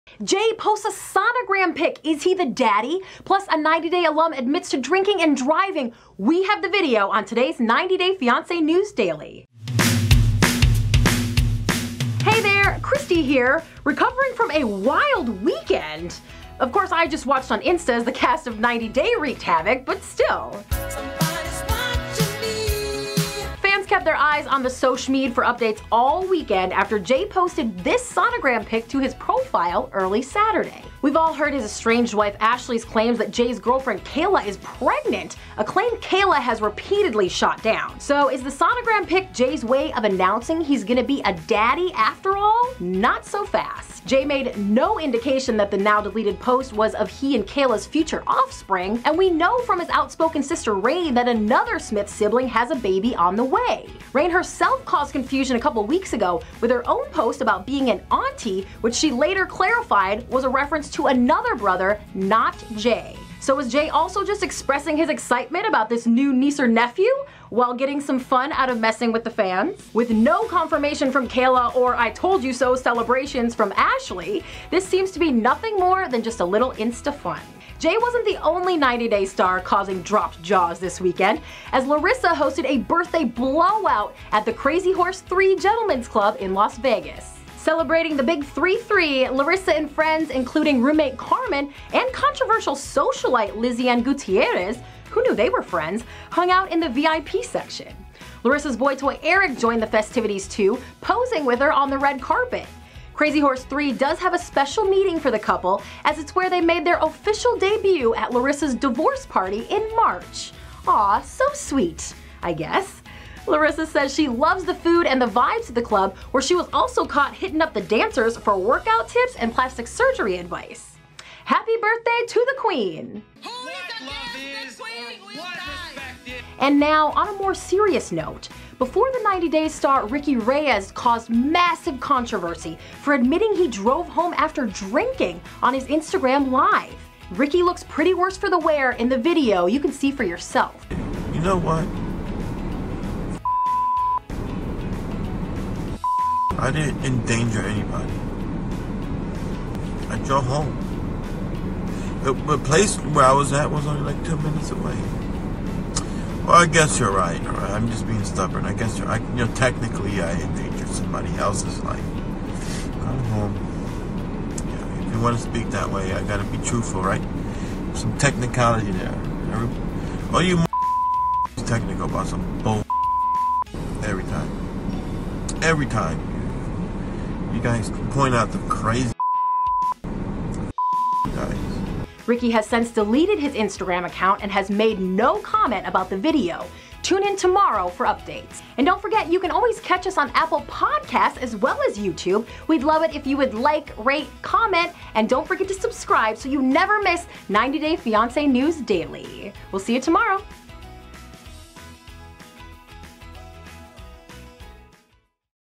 VIDEO: 90 Day Fiancé Star Admits To Drinking and Driving!!
Larissa turns 33, Jay posts a sonogram photo, but is he the daddy? Plus which 90 Day star admitted to driving after drinking? Christy Olson tells us all about it on today's 90 Day Fiancé News Daily!